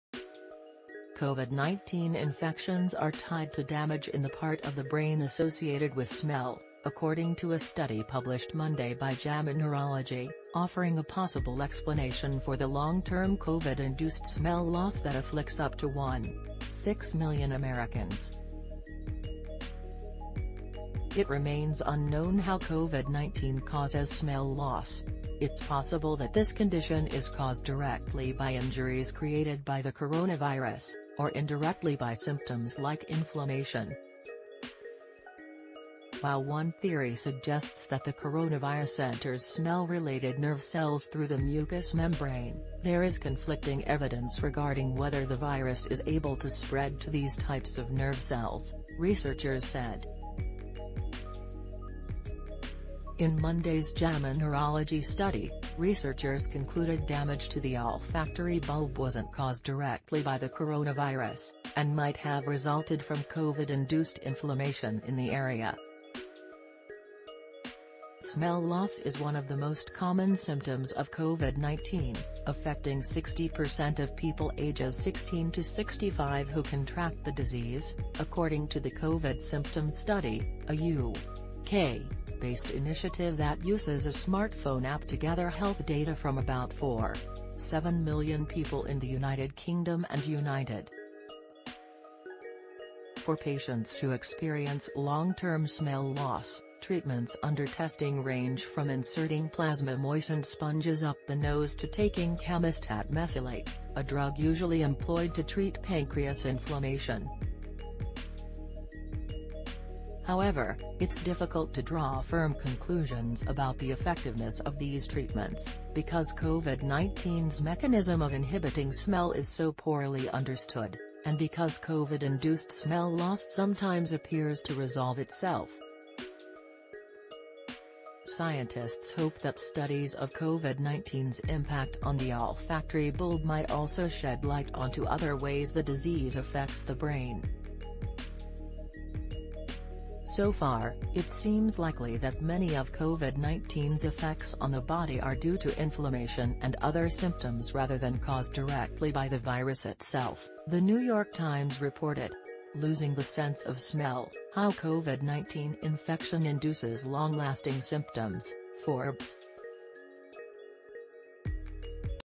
Covid Smell Loss Linked To Damage In Brain Study Finds
Covid-19 infections are tied to damage in the part of the brain associated with smell, according to a study published Monday by JAMA Neurology, offering a possible explanation for the long-term Covid-induced smell loss that afflicts up to 1.6 million Americans. It remains unknown how Covid-19 causes smell loss. It’s possible that this condition is caused directly by injuries created by the coronavirus, or indirectly by symptoms like inflammation. While one theory suggests that the coronavirus enters smell-related nerve cells through the mucous membrane, there is conflicting evidence regarding whether the virus is able to spread to these types of nerve cells, researchers said. In Monday’s JAMA Neurology study, researchers concluded damage to the olfactory bulb wasn’t caused directly by the coronavirus, and might have resulted from Covid-induced inflammation in the area. Smell loss is one of the most common symptoms of Covid-19, affecting 60% of people ages 16 to 65 who contract the disease, according to the COVID Symptom Study, a U. K.-based initiative that uses a smartphone app to gather health data from about 4.7 million people in the United Kingdom and United States. For patients who experience long-term smell loss, treatments under testing range from inserting plasma-moistened sponges up the nose to taking camostat mesylate, a drug usually employed to treat pancreas inflammation. However, it’s difficult to draw firm conclusions about the effectiveness of these treatments, because Covid-19’s mechanism of inhibiting smell is so poorly understood, and because Covid-induced smell loss sometimes appears to resolve itself. Scientists hope that studies of Covid-19’s impact on the olfactory bulb might also shed light onto other ways the disease affects the brain. So far, it seems likely that many of Covid-19’s effects on the body are due to inflammation and other symptoms rather than caused directly by the virus itself, the New York Times reported.“Losing the Sense of Smell: How Covid-19 Infection Induces Long-Lasting Symptoms” (Forbes)